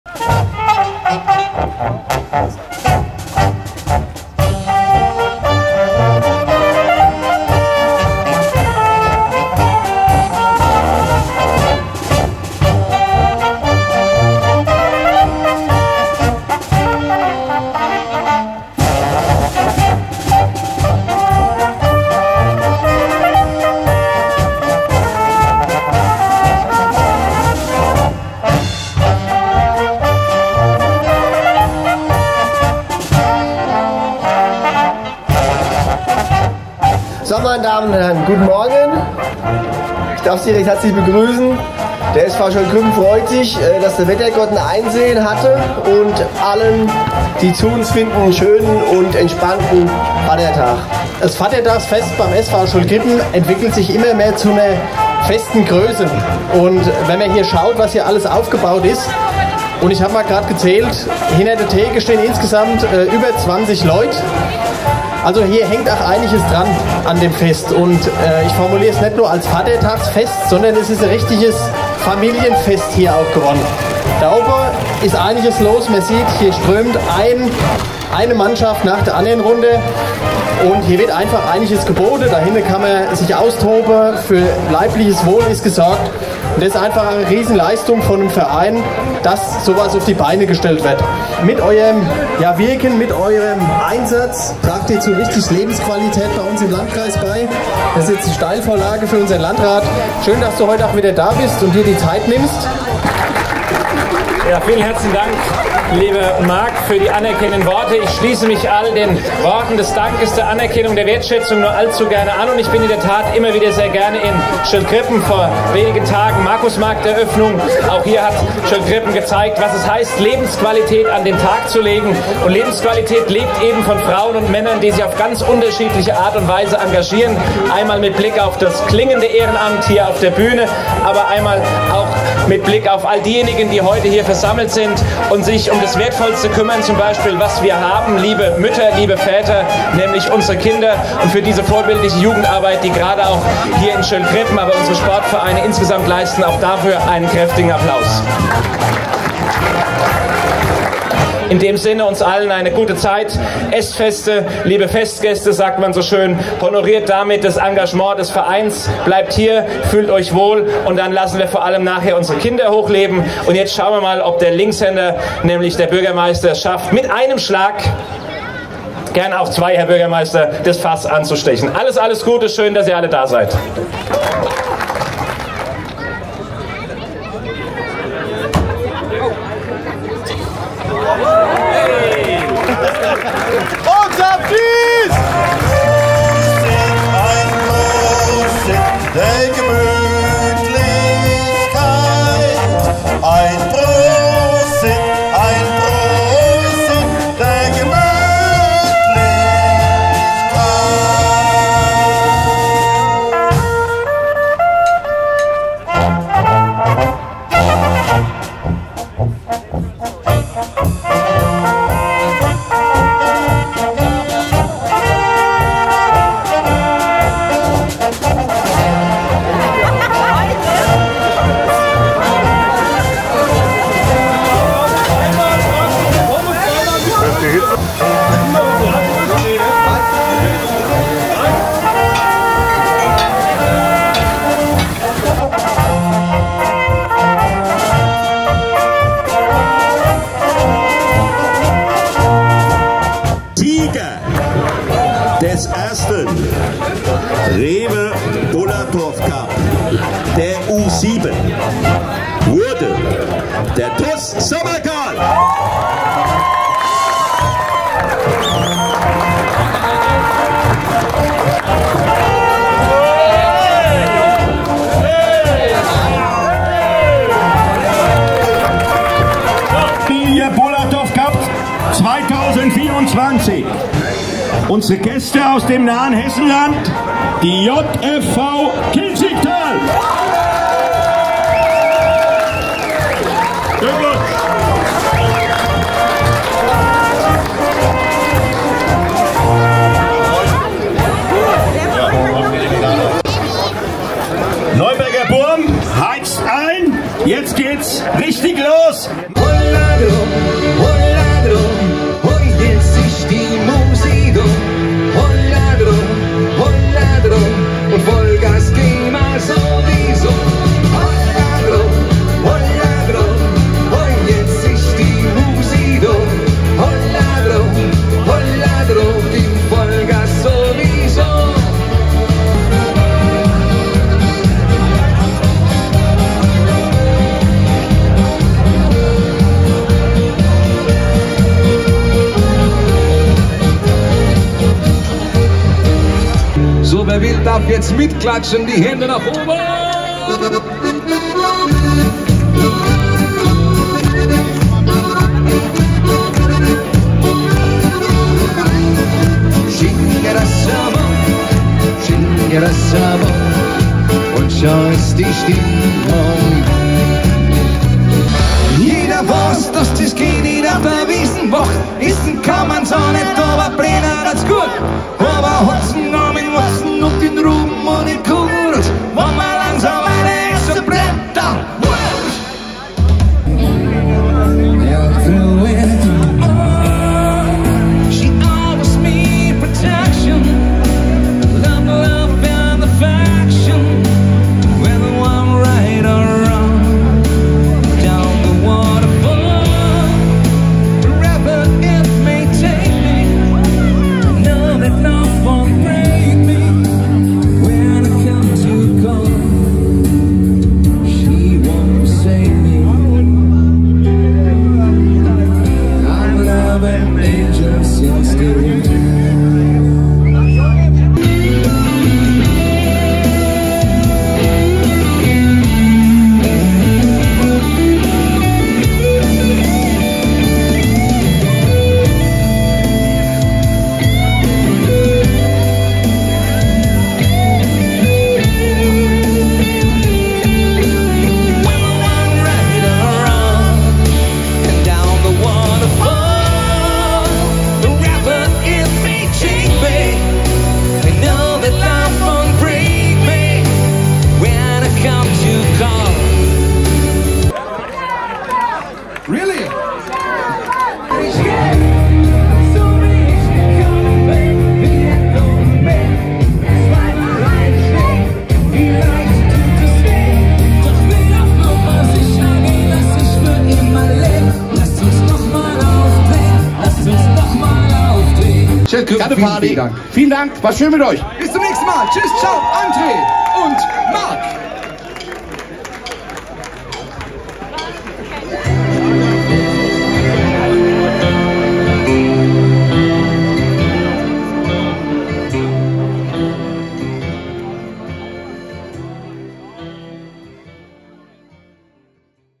SV Schöllkrippen Vatertag 2024
Der SV Schöllkrippen hat auch dieses Jahr am Vatertag, den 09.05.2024, zum Vadderdoachsfest am Höllenbach (auf dem Parkplatz des SV Schöllkrippen) eingeladen. Ein großartiges Rahmenprogramm mit Unterhaltung für die ganze Familie!
Bieranstich mit unserem Bürgermeister Marc Babo und unserem Landrat Dr. Alexander Legler, Blasmusik mit den Kahlgründer Musikanten,  Burger vom Biorind und Bratwürste vom Berghof und weitere Leckereien wie Käsestangen und Pommes. NEU: Partyhits von den Neuberger Buam